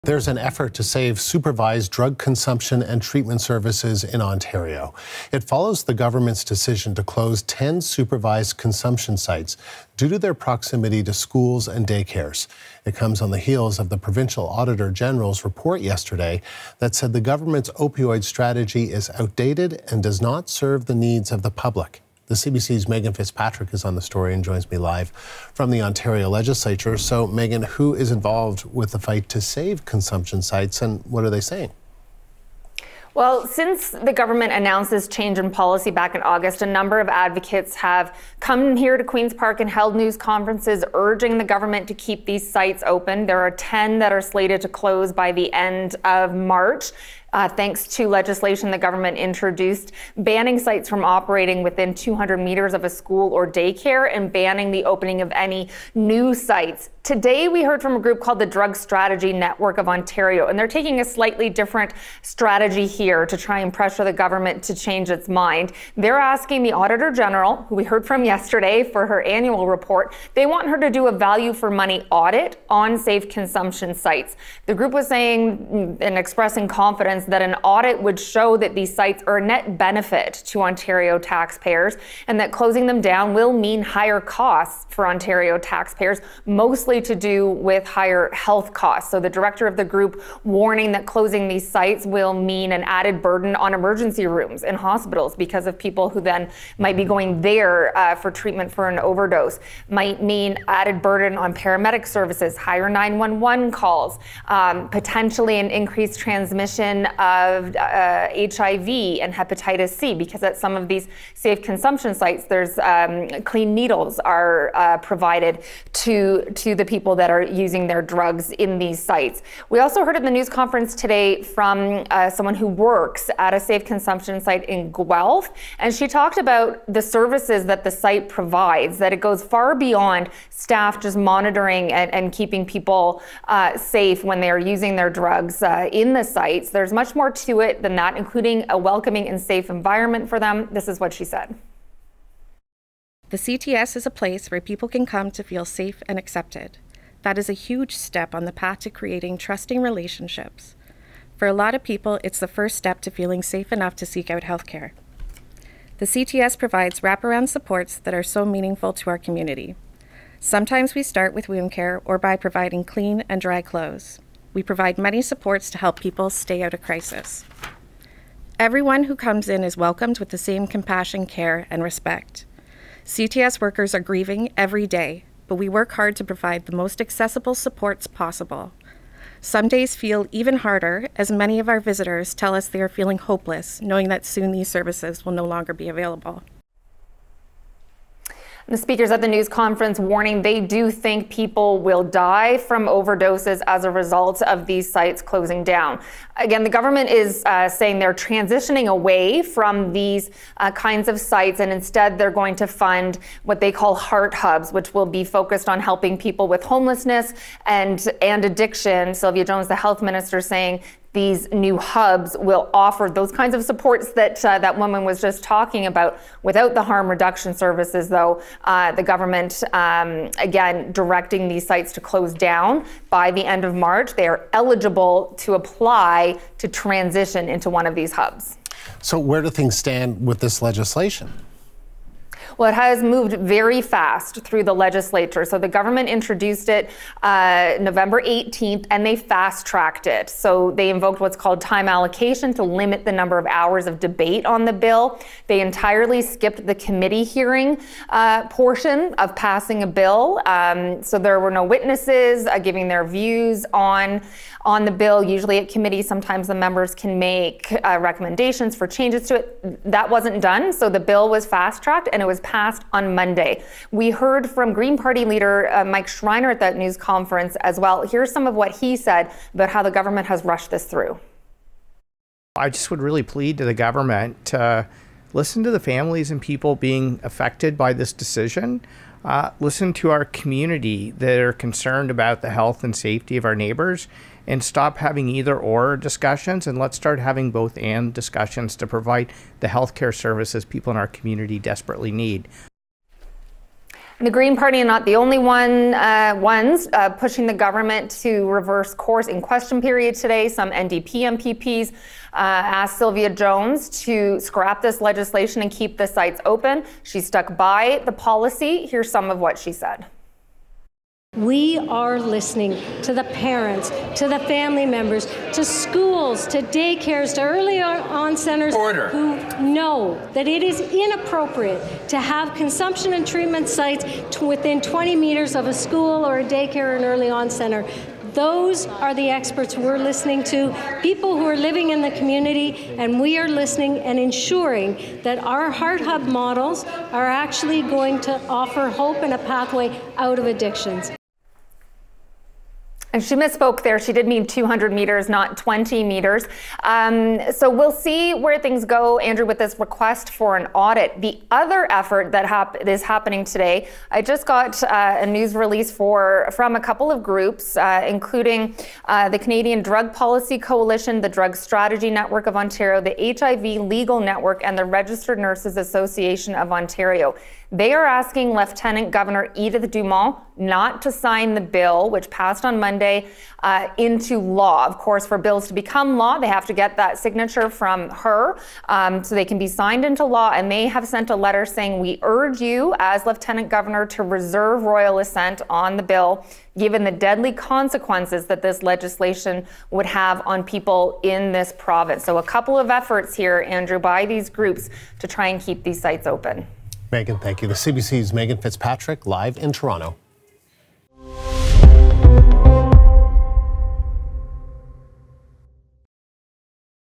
10 supervised drug consumption sites in Ontario set to close by March 2025
There is a push to save 10 supervised drug consumption sites in Ontario that are set to be closed because of their close proximity to schools and daycares. Health workers, advocates and users of the sites have warned of a spike in deaths when the sites close by March 31, 2025.